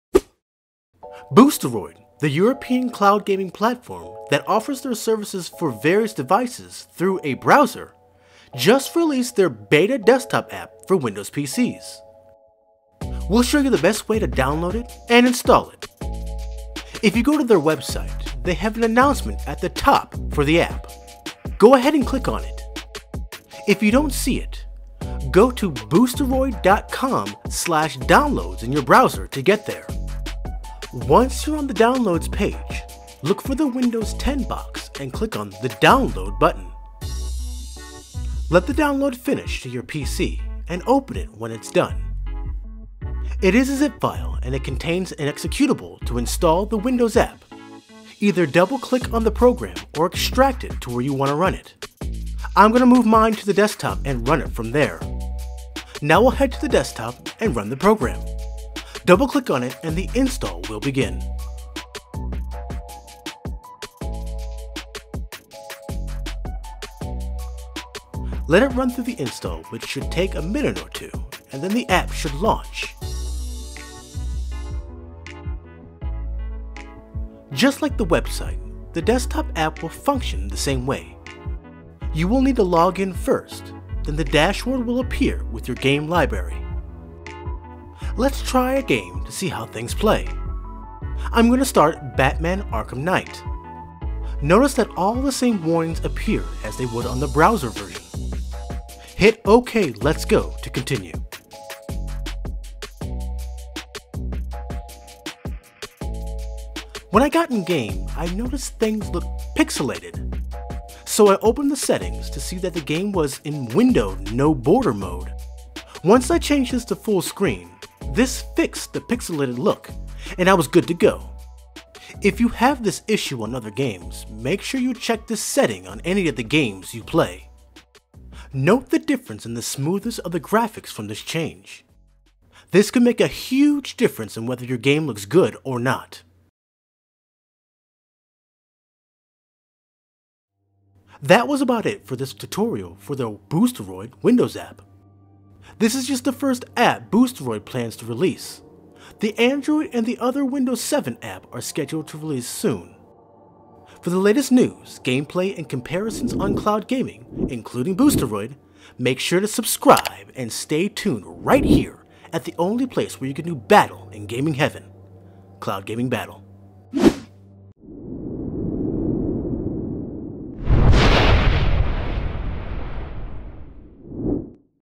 BOOSTEROID | Install DESKTOP APP Beta
Boosteroid just released their Beta Desktop app for Windows PCs. We’ll show you the best way to download it and install it.